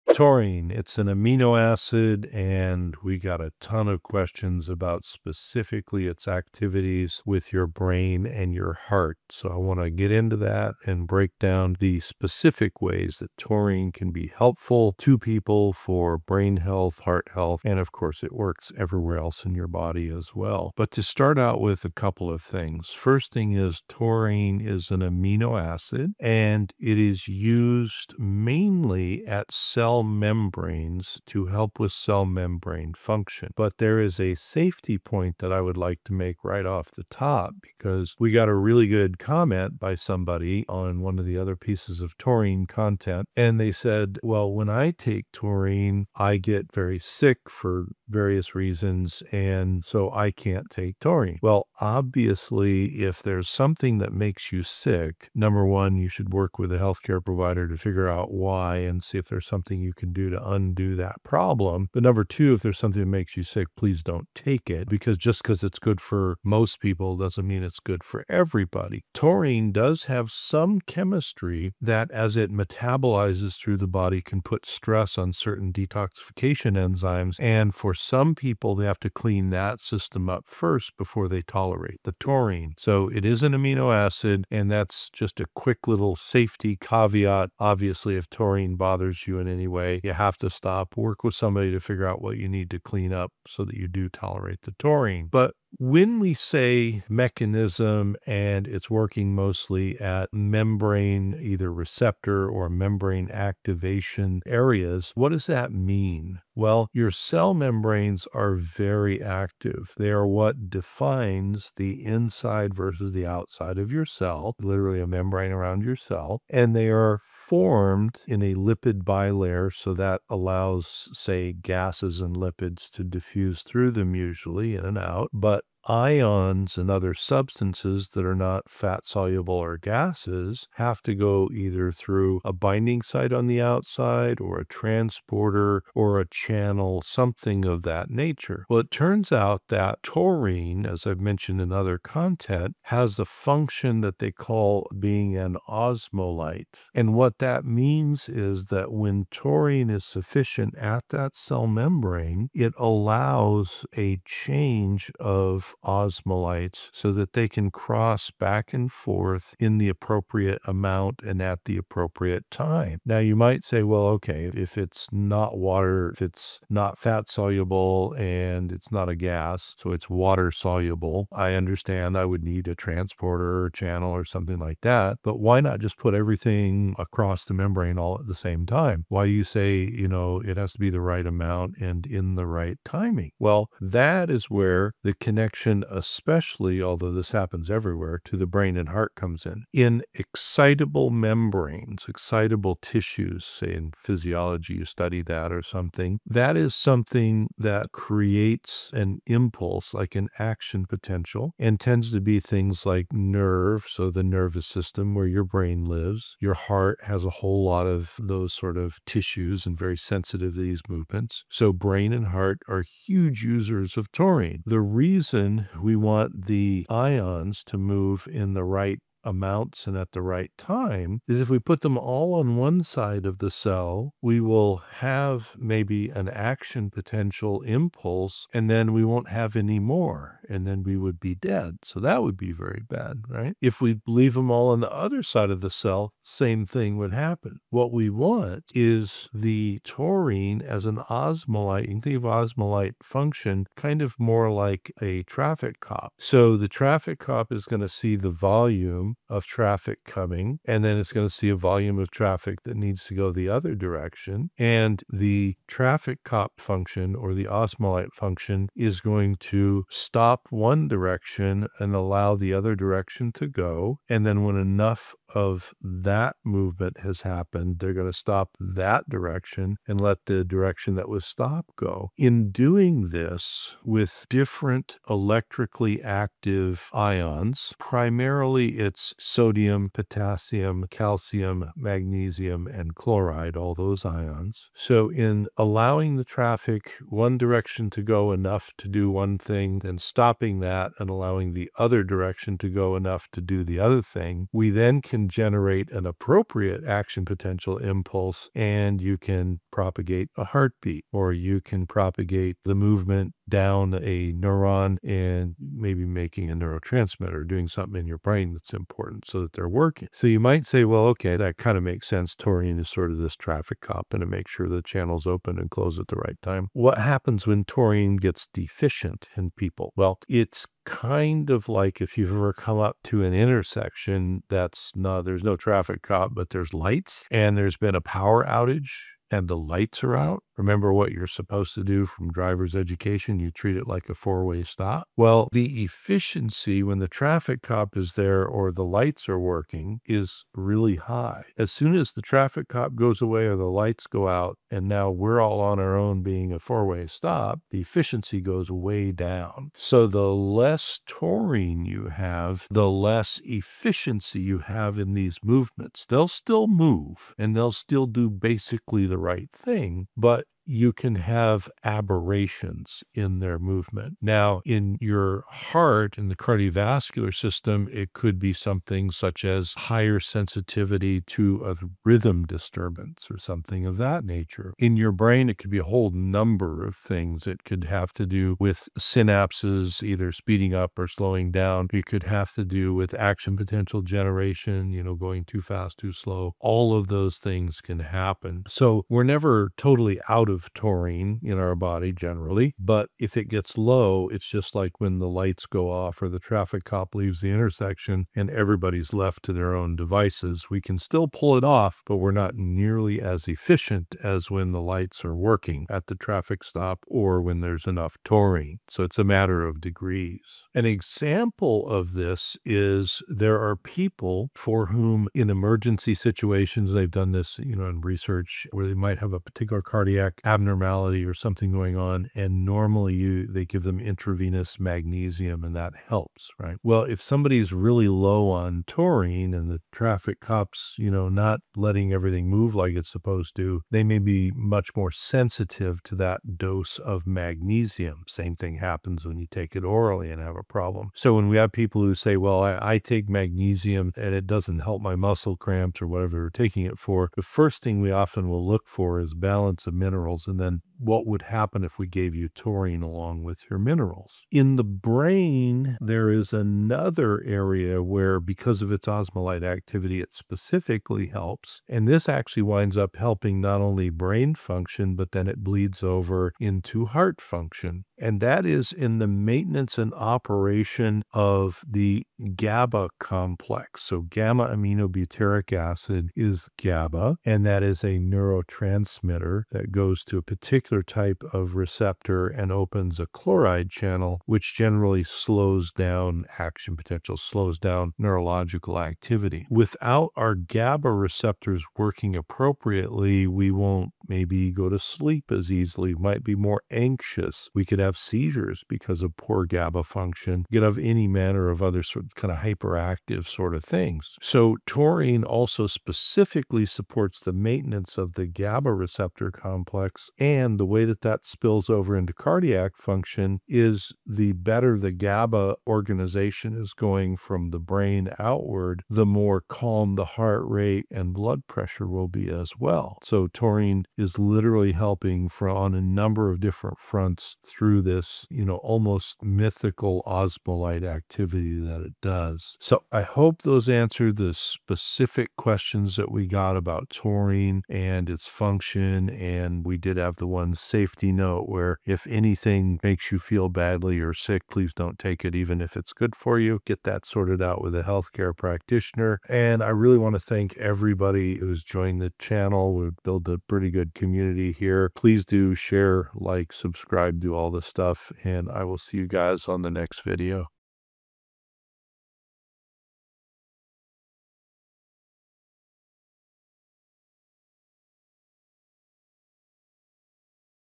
The Incredible Impacts of TAURINE on Your Brain and Heart
This video explains how taurine, an amino acid, supports your brain and heart health. Learn about the science behind what is taurine and it's many health benefits, and discover what happens when brain supplements are needed to boost your brain power.

CHAPTERS

00:00 Taurine Overview + Why It Matters
00:26 What Taurine Does at the Cell Level
00:35 Safety Warning: When Taurine Might Make You Sick
01:15 Taurine’s Role in Detox Pathways
01:38 Taurine and Cell Membrane Function Explained
02:21 What “Osmolite” Means and Why It’s Important
03:08 Why Brain and Heart Depend on Taurine
04:01 Taurine as the “Traffic Cop” of Your Cells
04:43 Key Ions Taurine Regulates (Na, K, Mg, Ca, Cl)
05:02 Taurine and Action Potentials in Heart and Brain
06:05 What Happens When Taurine is Deficient
07:03 Magnesium Sensitivity: A Clue You’re Low in Taurine
07:36 Taurine’s Role in GABA and Brain-Calming Effects
08:36 Taurine's Effect on Heart Rate and Blood Pressure
08:49 Summary: Why Taurine Is a Foundational Nutrient
09:03 Final Safety Note + Channel Outro

Helpful Links